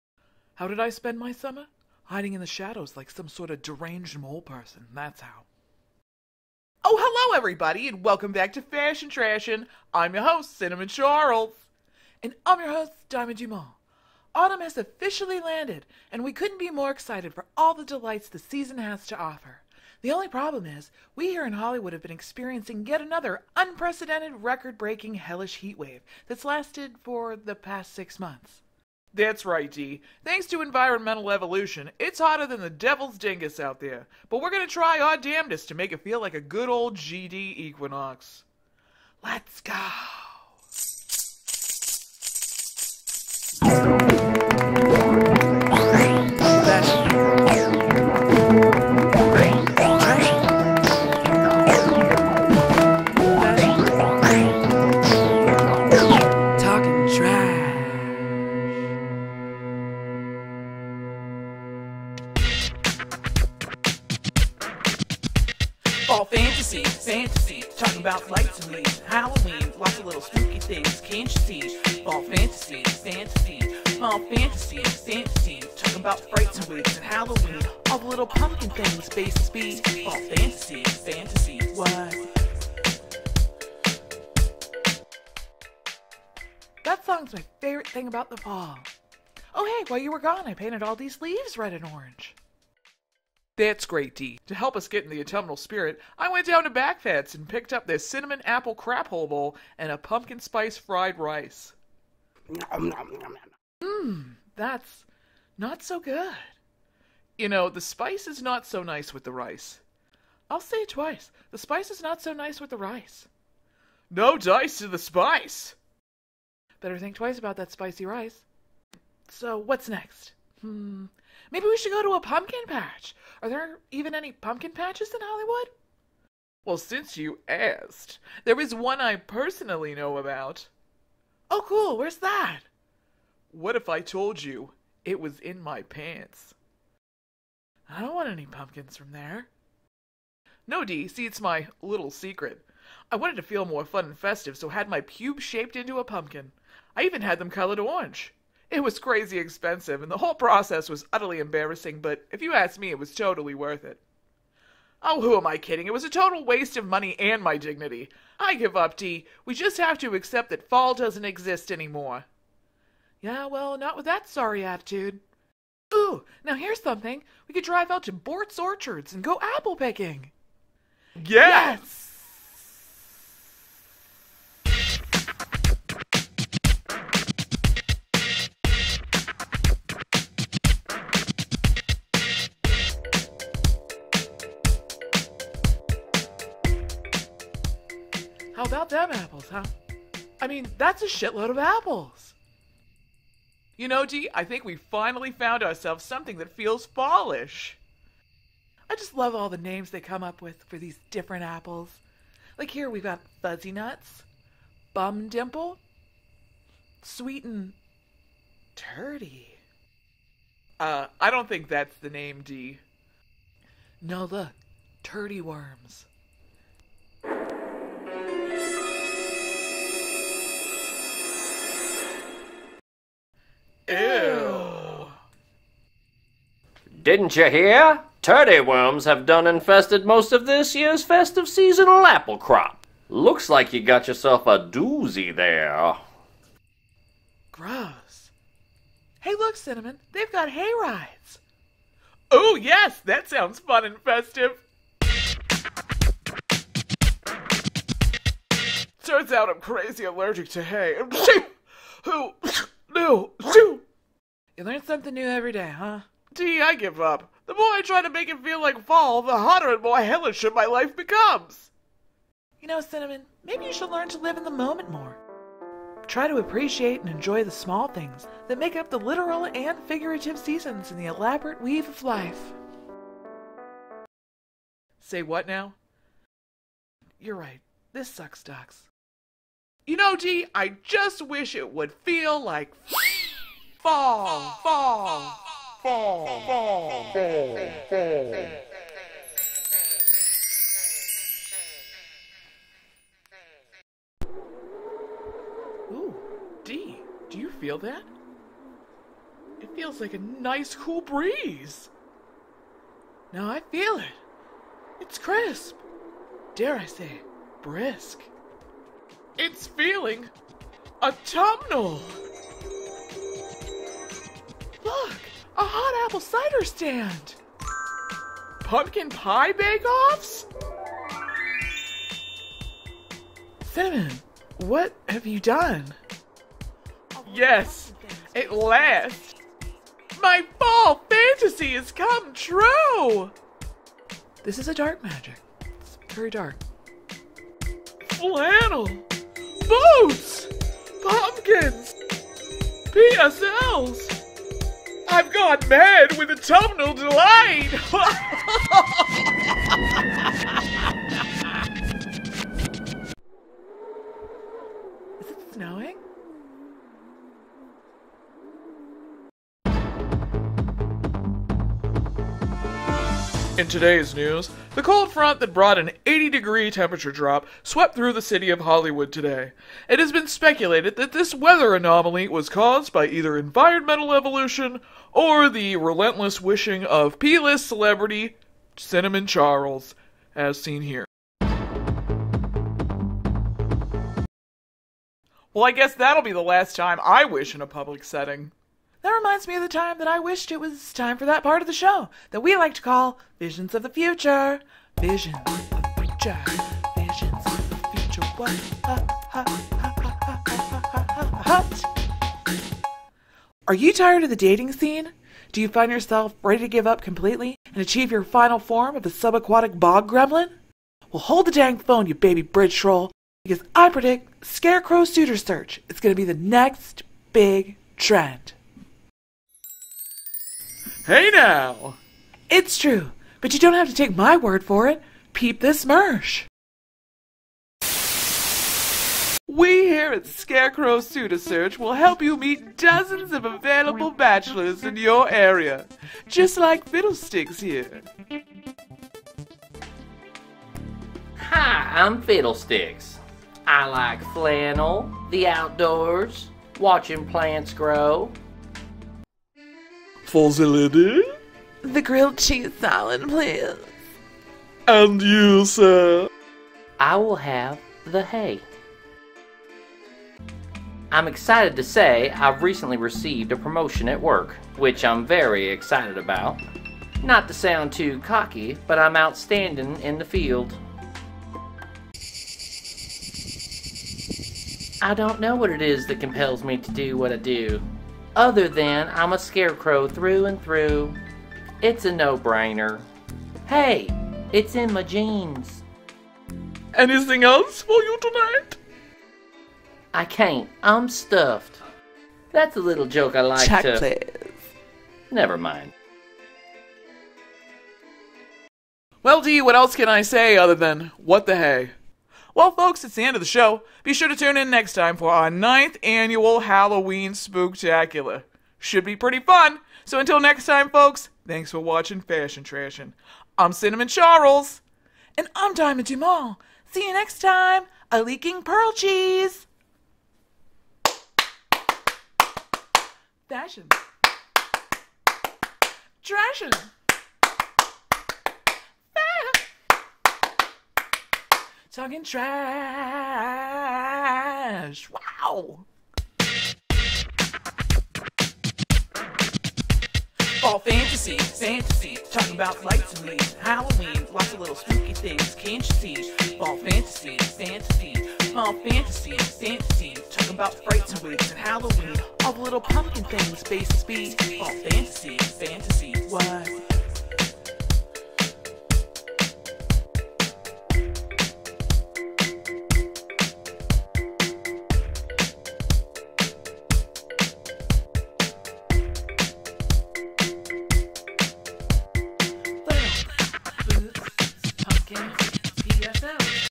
Fashion Trashin 92; Fall Fantasy 🍂 || Flannel! Boots! Pumpkins! PSLs!
Cinnamon and Diamond find themselves relentlessly attempting to engage in autumnal activities.

Find us across the internets.

Download some original tunes.